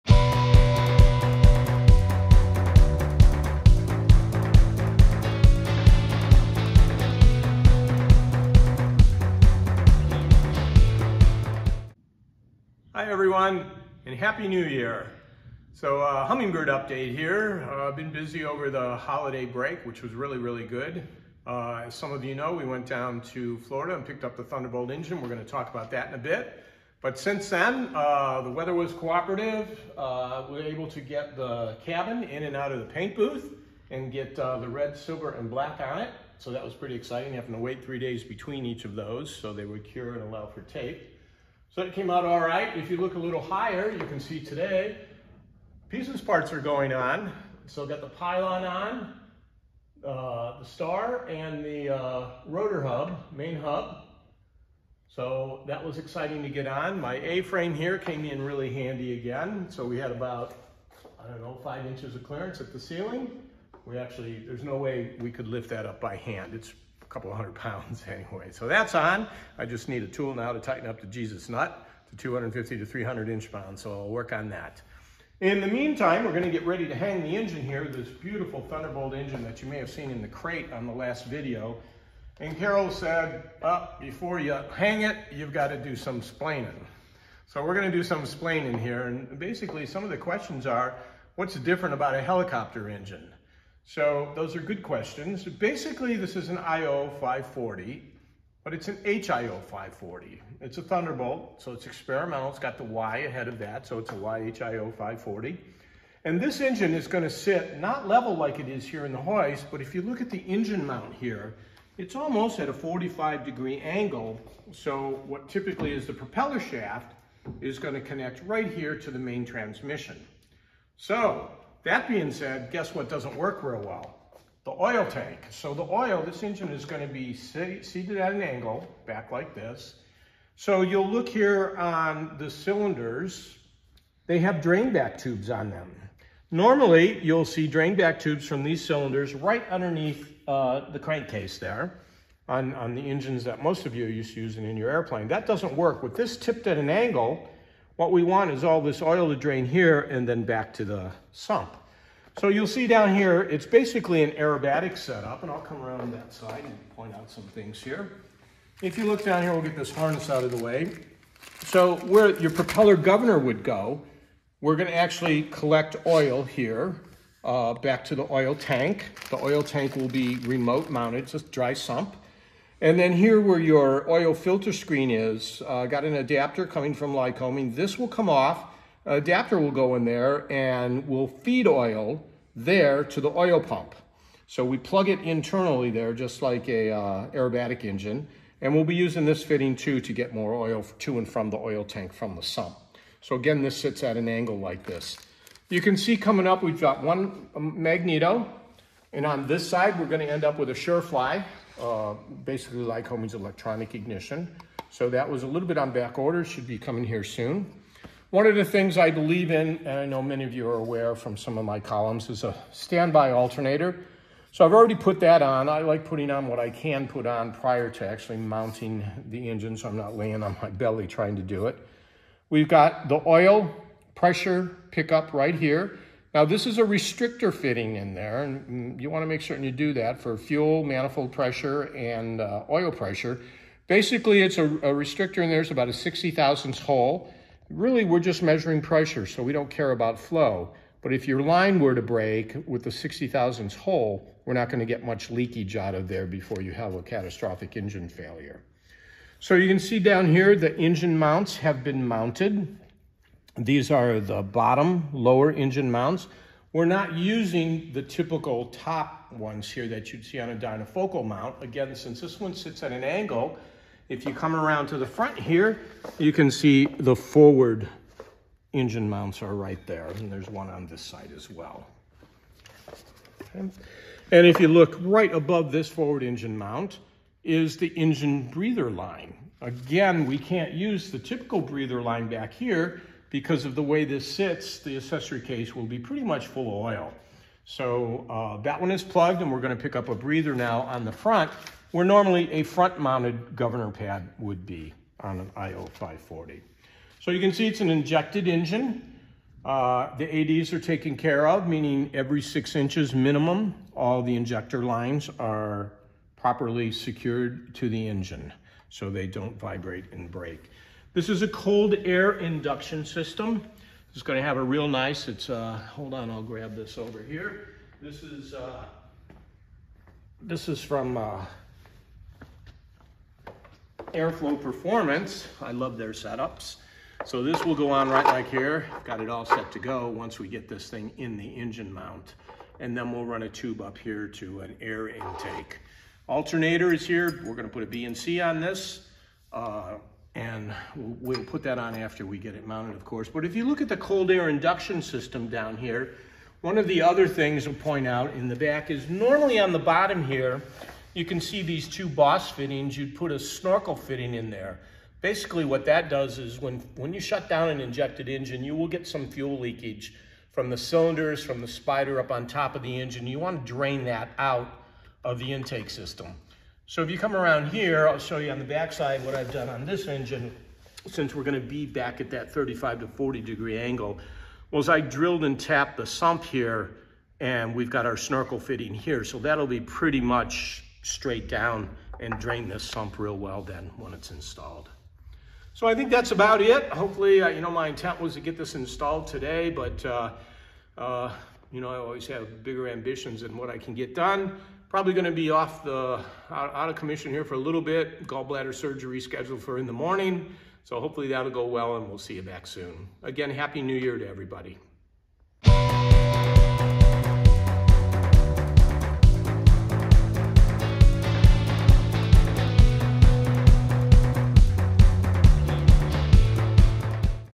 Hummingbird Update-The Thunderbolt Engine
Vic from Base Leg Aviation discusses the differences between a helicopter IO540 and airplane IO540.

The snorkel fitting mentioned in the video is a one way valve. It allows fuel to drain from the intake sump when the engine is shut down. It has a little ball in it that closes the valve when the engine is running due to the vaccum in the sump. Part number from lycoming is 75444. Van's aircraft carries them.